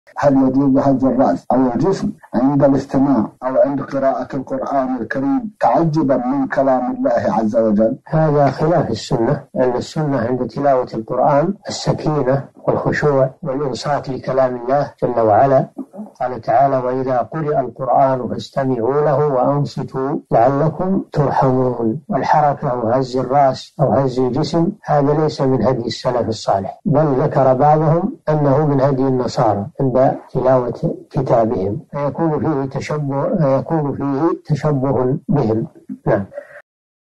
هل يجوز هز الرأس أو الجسم عند الاستماع أو عند قراءة القرآن تعجبا من كلام الله عز وجل؟ صالح الفوزان
نص السؤال: هل يجوز هزّ الرأس أو الجسم عند الاستماع أو عند قراءة القرآن الكريم تعجُّبًا من كلام الله عز وجل؟

السيرة الذاتية :
للاشتراك في قناة الشيخ صالح الفوزان

رئيس الفريق
لمشروع كبار العلماء